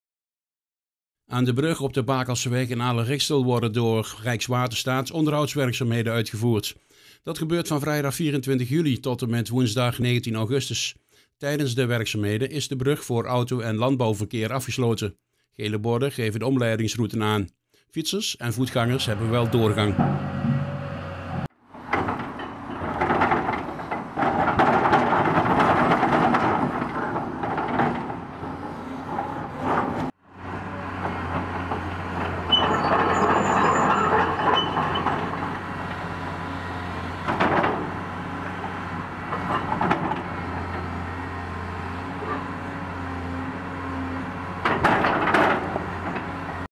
27-07-2020 Werkzaamheden aan brug Bakelseweg
Aan de brug op de Bakelseweg in Aarle-Rixtel worden door Rijkswaterstaat onderhoudswerkzaamheden uitgevoerd. Dat gebeurt van vrijdag 24 juli tot en met woensdag 19 augustus Tijdens de werkzaamheden is de brug voor auto- en landbouwverkeer afgesloten. Gele borden geven de omleidingsroute aan. Fietsers en voetgangers hebben wel doorgang.

Camera: Gerrit Otten
Montage: Martien van de Vossenberg
Voice-over: Hans van Erp